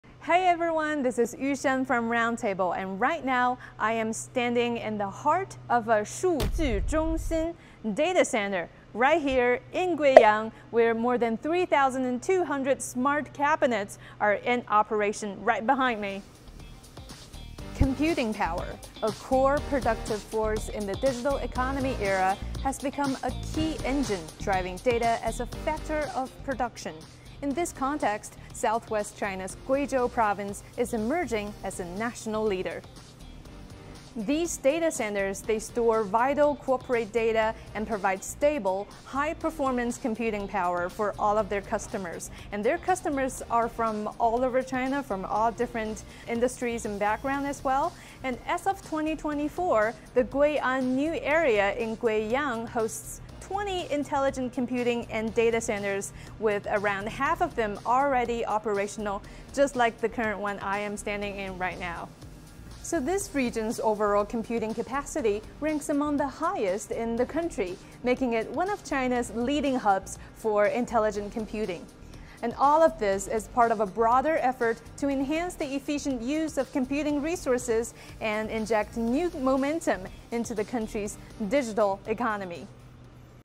Guiyang's data powerhouse
Step inside the tech that's shaping our future!  With 3,200+ smart cabinets and top-tier computing power, data centers like this one in Guiyang, southwest China's Guizhou Province, are fueling the nation's digital future. Join reporter Yushan to find out more!